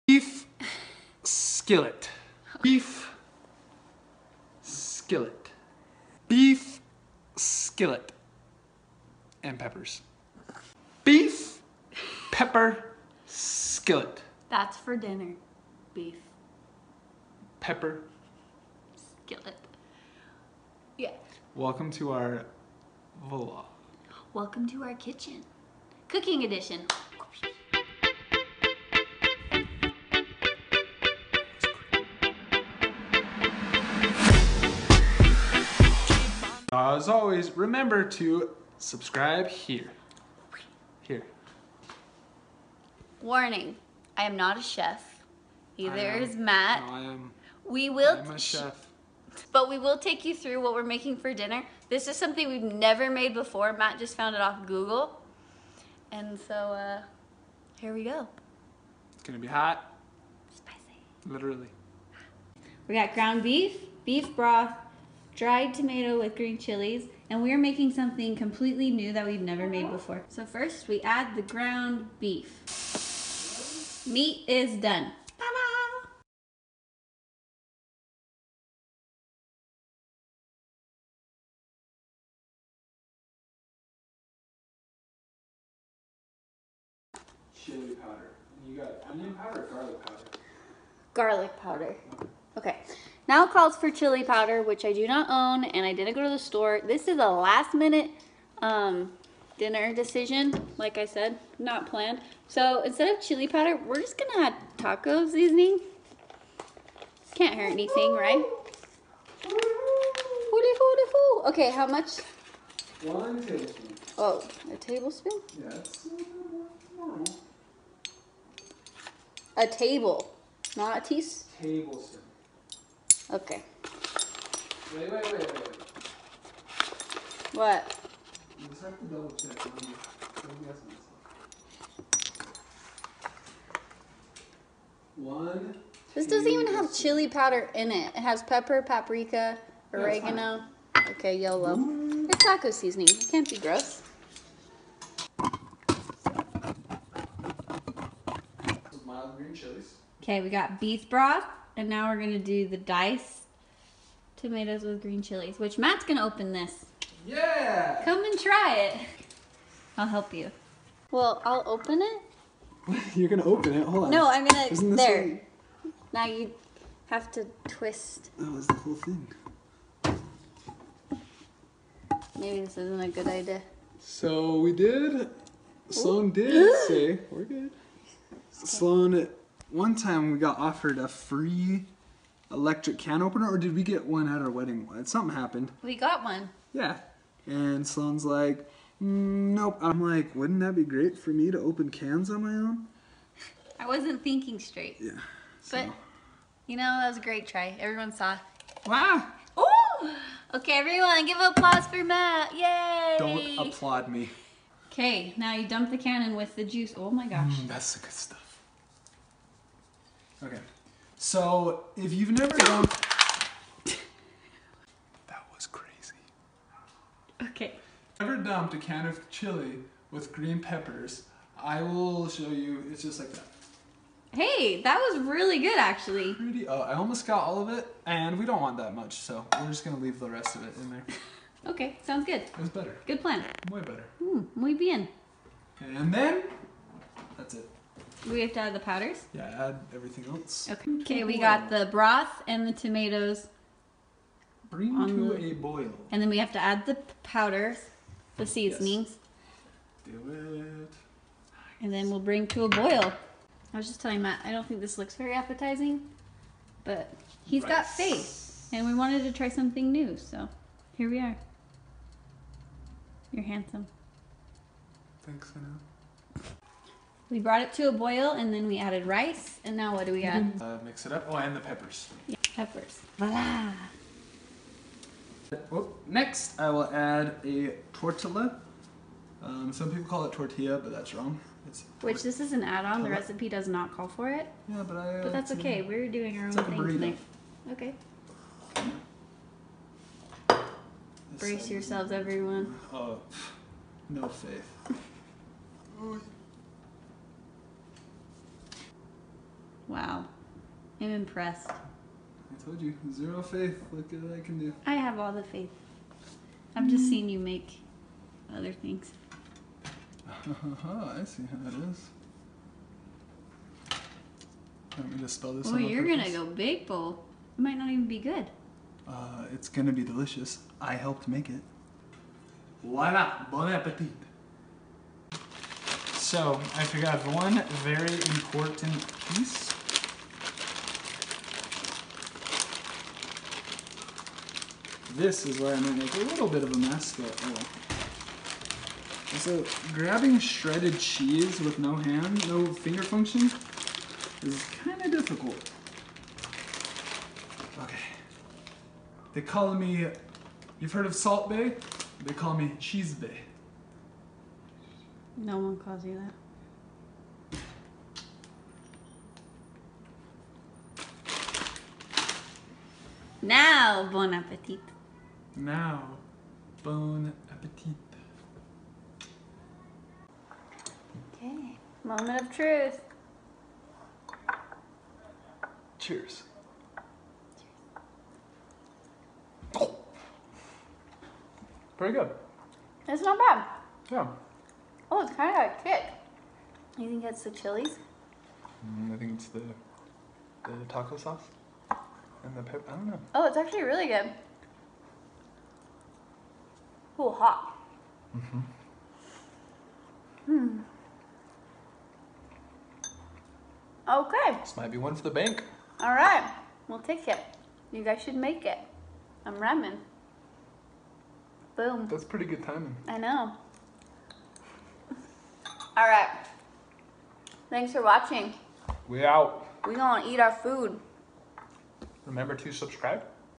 Matt & Sloan-in the Kitchen
Making dinner for date night. We found a random recipe and decided to try something new. Matt's a chef. Watch his skill and see how it turned out.